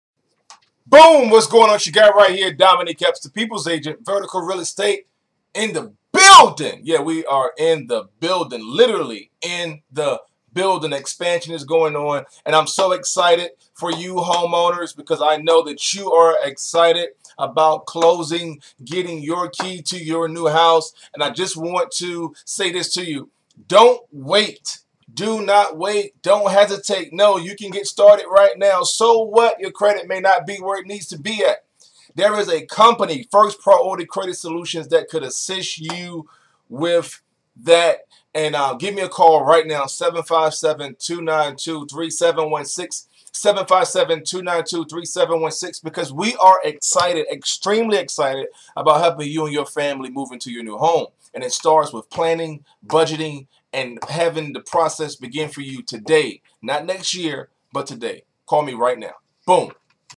Hi Homeowners! Start Now!!!
Hi Homeowners,

I am Domenick Epps, REALTOR with Vertical Real Estate (formerly Benham Real Estate Group of Virginia) and I am excited for you and with you in your journey in becoming a homeowner.  Here's the thing, YOU CANNOT AFFORD TO WAIT until everything is lined up.  Truthfully Underwriting will show you how much you don't have everything lined up, lol.

But start the process today.  Credit issues, no problem, lets get going today and get you where we know you need to be but it starts with making one simple phone call to you guess it, ME.

DOMENICK EPPS, REALTOR
VERTICAL REAL ESTATE